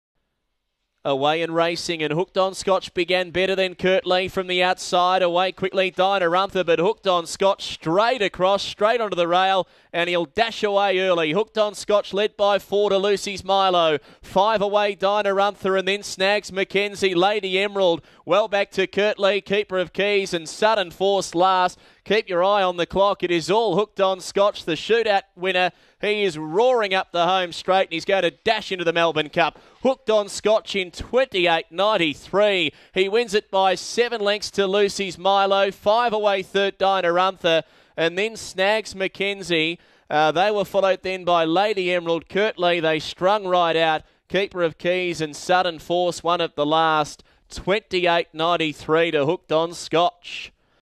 Hooked On Scotch sets a record breaking 28.94
Hooked On Scotch (8) stuns the racing world by setting a new track record in his heat of 2019 TAB Melbourne Cup - Friday 22 November, 2019.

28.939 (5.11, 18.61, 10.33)